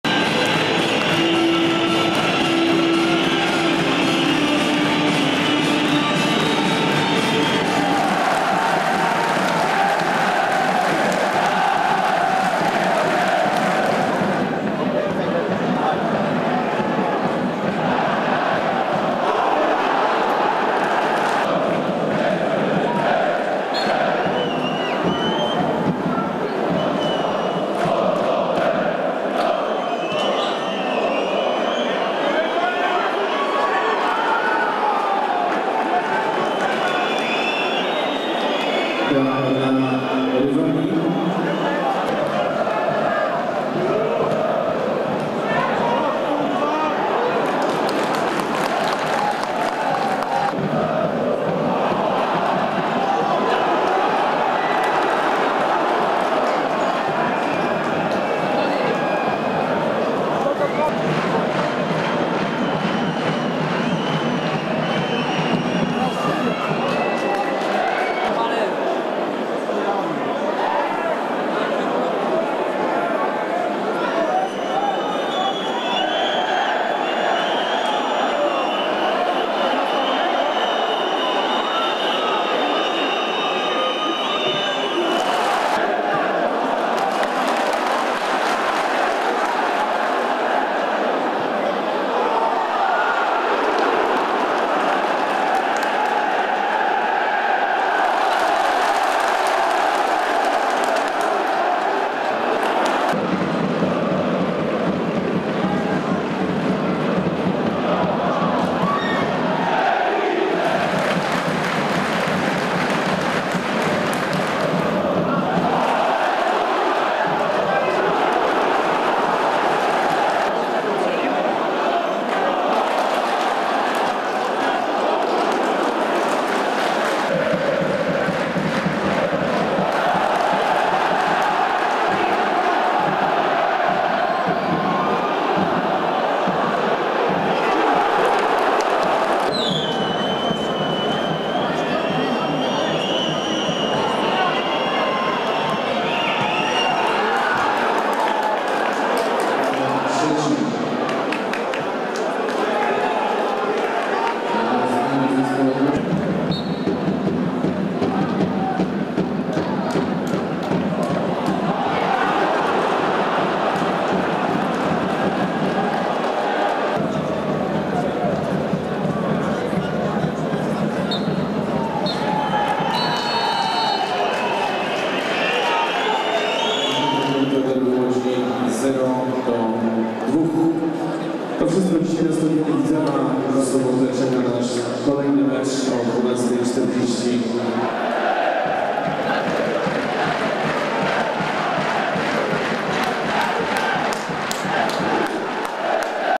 65. Derby Łodzi: Widzew - ŁKS 0:2 | SKRÓT MECZU
2. kolejka Fortuna 1 Ligi
Widzew Łódź – ŁKS Łódź 0:2 (0:1)

Bramki: 0:1 Maksymilian Rozwandowicz (16), 0:2 Adrian Klimczak (64)